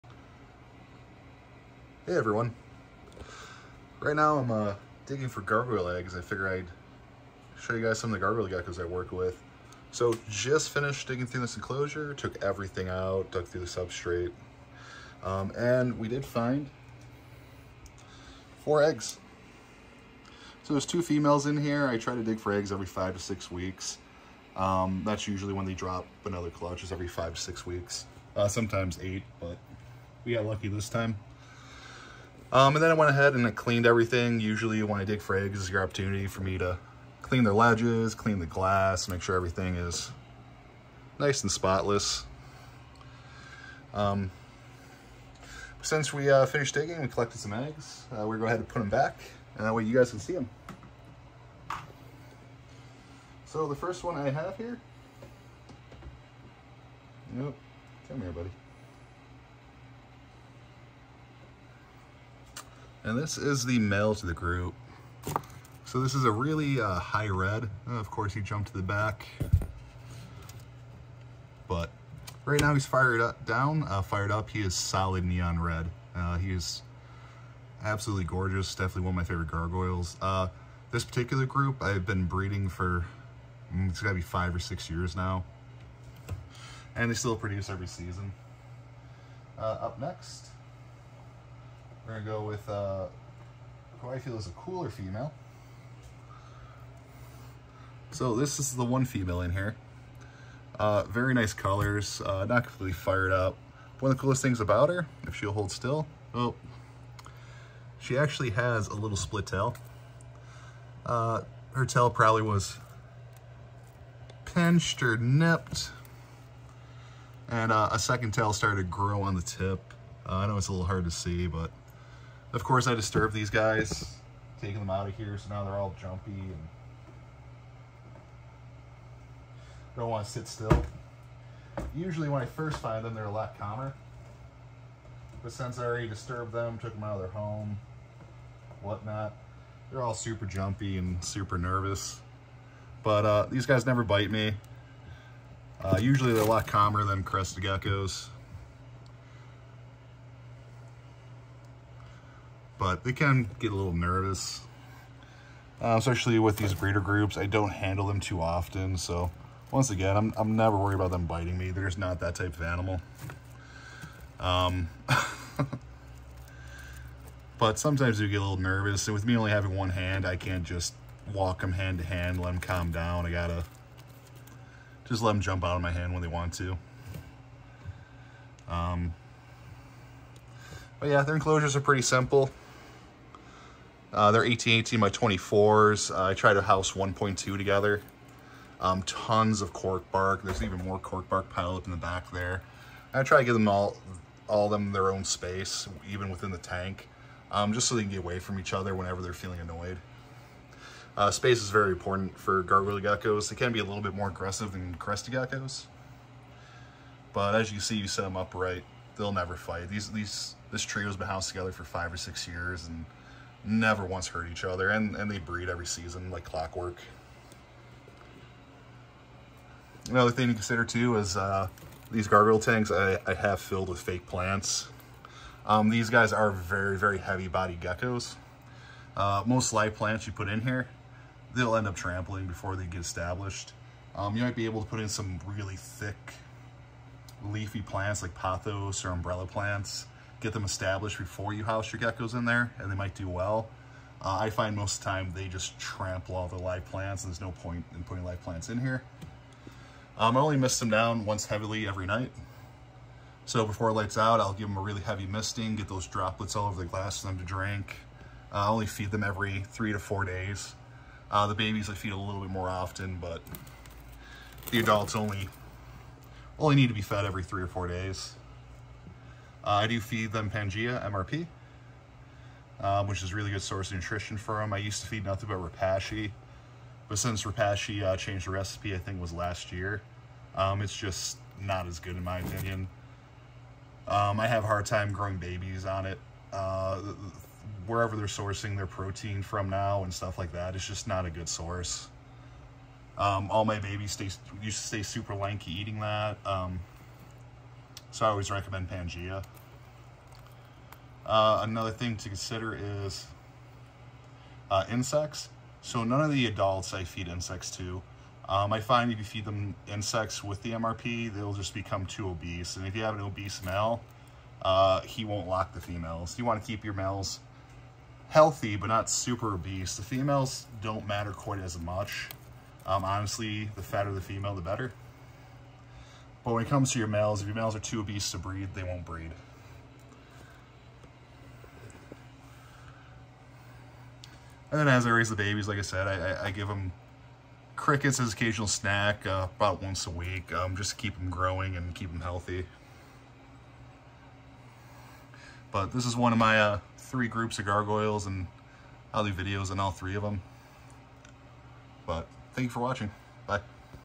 Gargoyle gecko breeding. Eggs collected, enclosure, diet and care
Gargoyle gecko breeding groups. Meet my gargoyles part 1 of 3. Care, enclosure, eggs and much more.